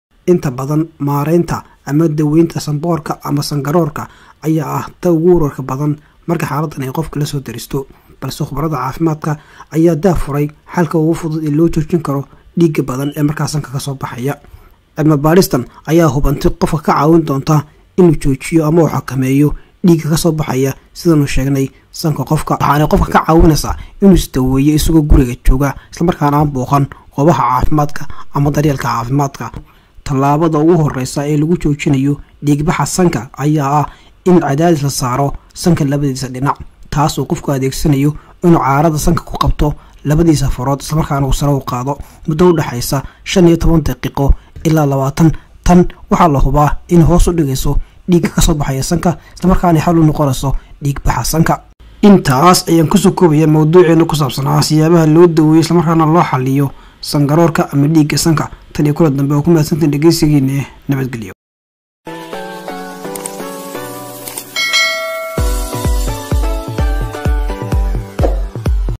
Sida La Isaga Daweeyo Dhakhtar La'aan San Gororka
Dhiigbaxa sanka habka ugu fudud ee la isaga daweeyo.